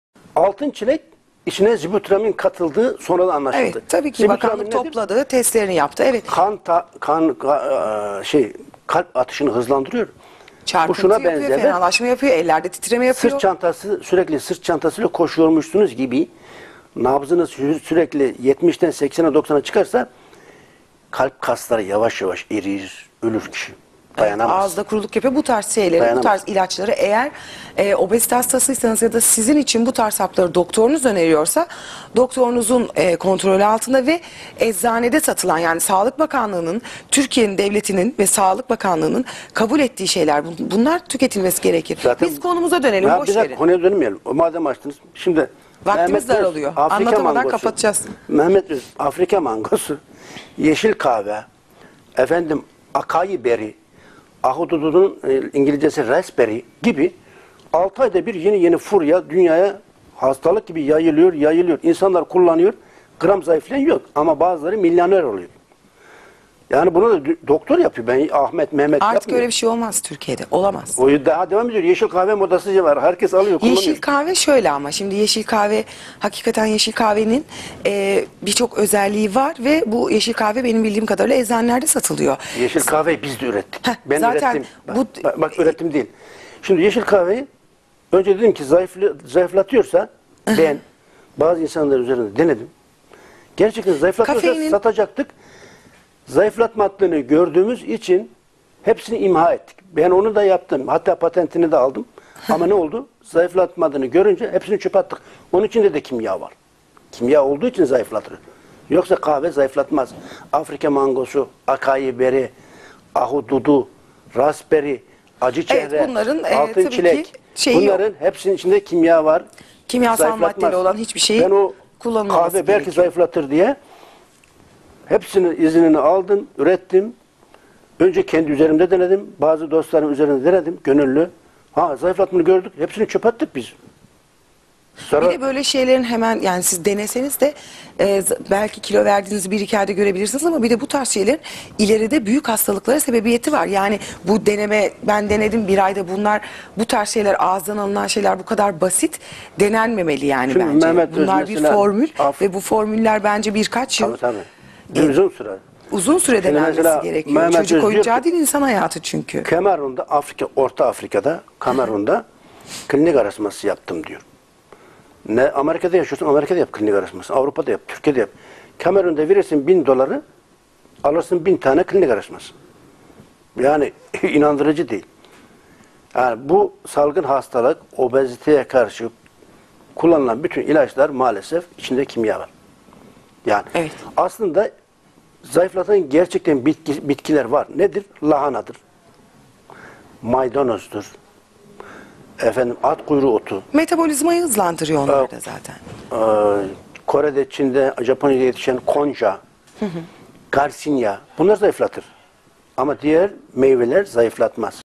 Mehmet Öz'den Masallar, Yeşil Kahve, Altın Çilek, Doğal Zayıflama, Şifa, Herbalist, Afrika Mangosu
Bitki Bilimci ve Araştırmacı Yazar İbrahim Gökçek Doğal Zayıflama, Mehmet Öz, Yeşil Kahve, Afrika Mangosu, Altın Çilek, Bitkisel Tedavi ile ilgili soruları cevaplıyor.